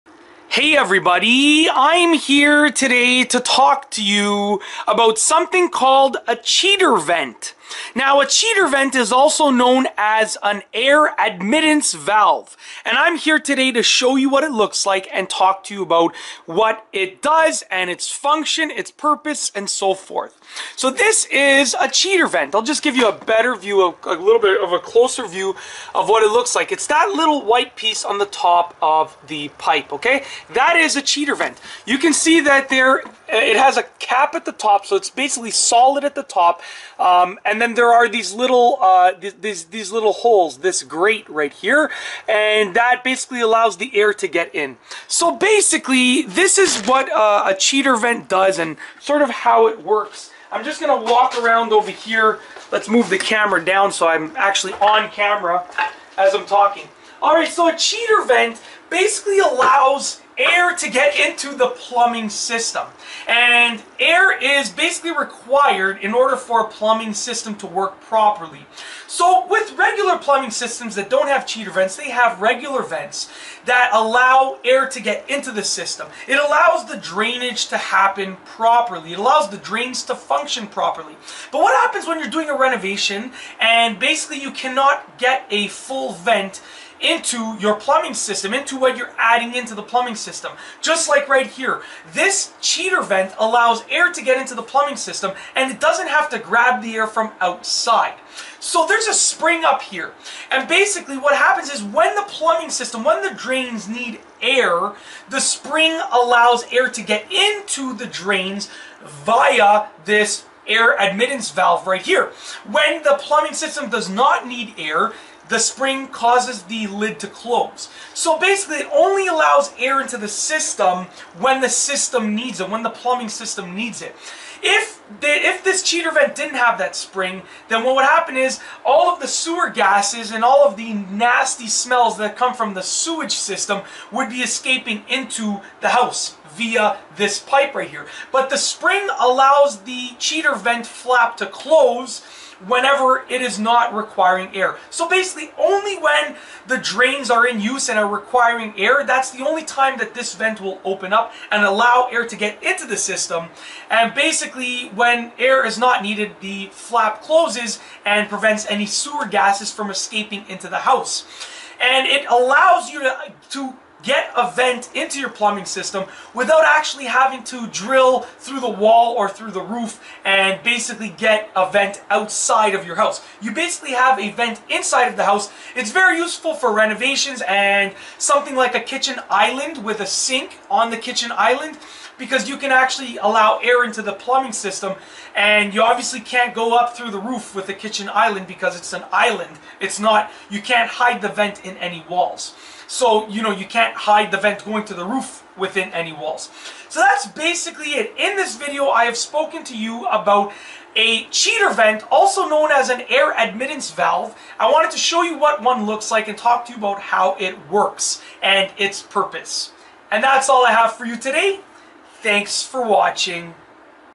Cheater Vent For Plumbing-How It Works (AKA Air Admittance Valve)
In this video, I talk about a cheater vent that is sometimes used in plumbing systems! A cheater vent is often referred to as an air admittance valve, and is used to allow air to enter the plumbing system without using a pipe to connect to the outside of the house. Cheater vents are often used in renovations or when incorporating a sink into a kitchen island. Air admittance valves are an easy way to let air into your drainage system (so that it functions properly).